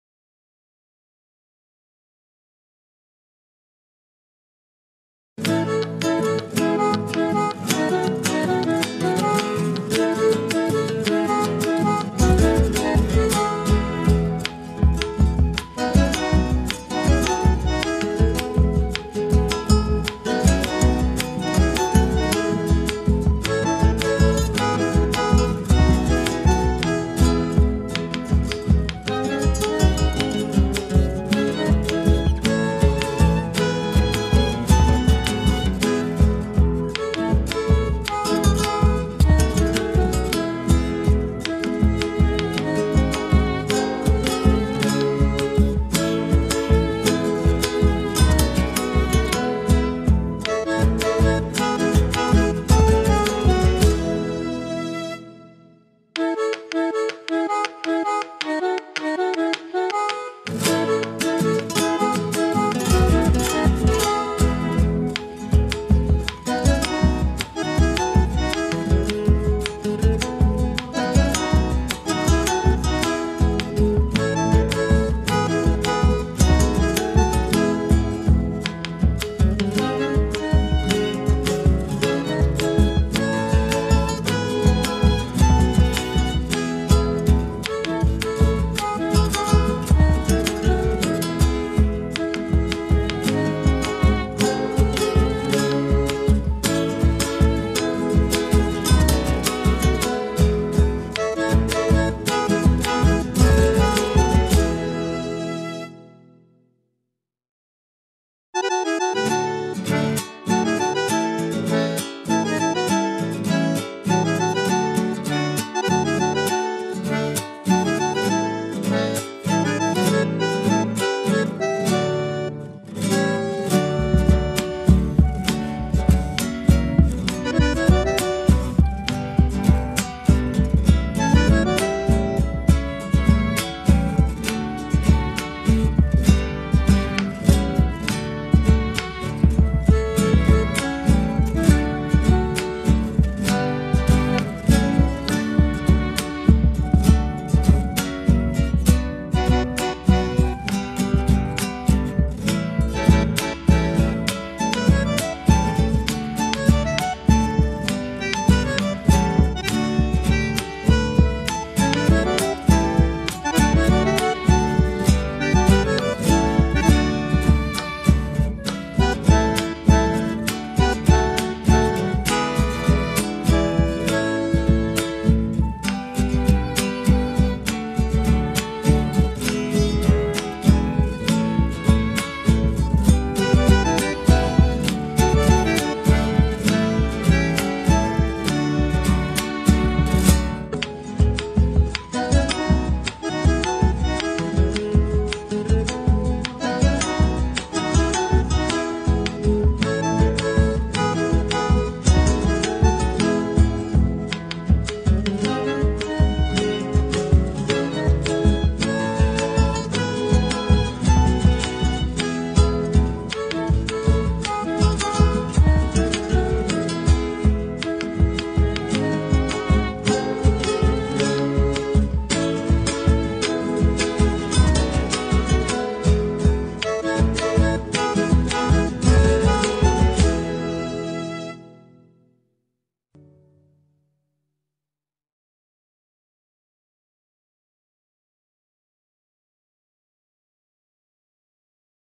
Historia de José de San Martín para niños.
Breve relato sobre la vida de José de San Martín acompañada con retratos de San Martín en distintas épocas  y  otras pinturas